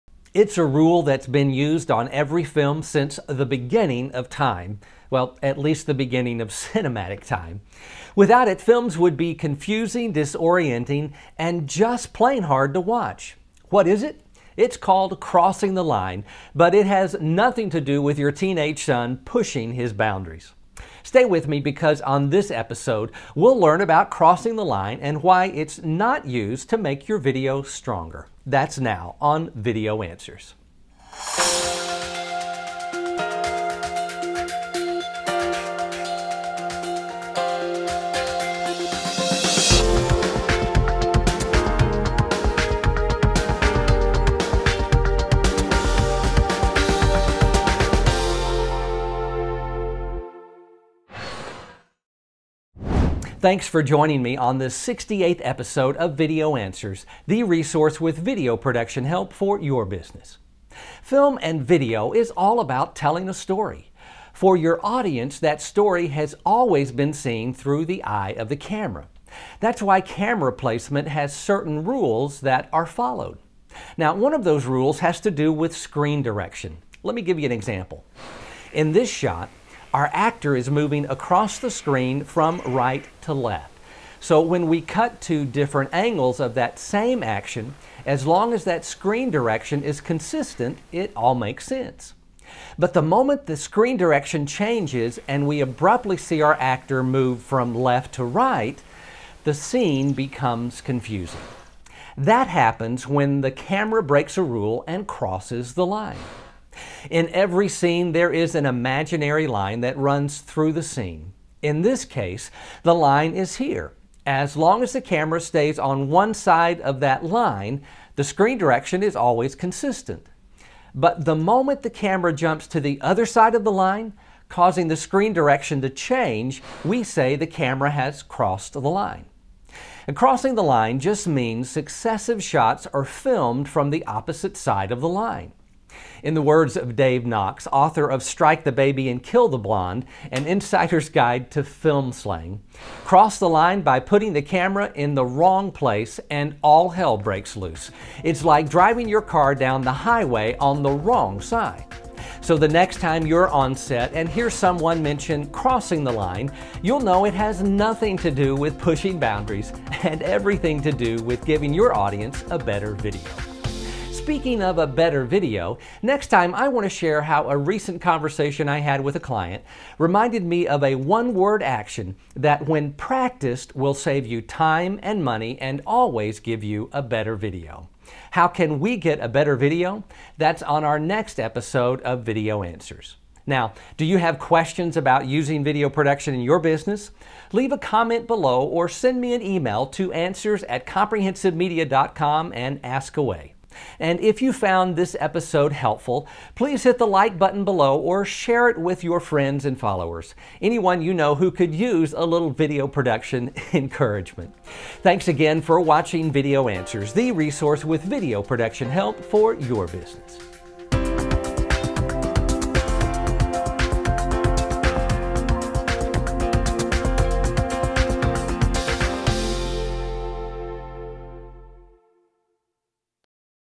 "What is Crossing the Line?" Video Answers. Episode 68.
It’s a rule that has been used on every film since the beginning of time - at least the beginning of cinematic time. Without it, films would be confusing, disorienting, and just plain hard to watch. What is it? It’s called Crossing the Line but it doesn’t have anything to do with your teenage son pushing the boundaries.

Stick around with me, because on this episode we'll learn about Crossing the Line and why it’s NOT used to make your video stronger. That’s now on Video Answers.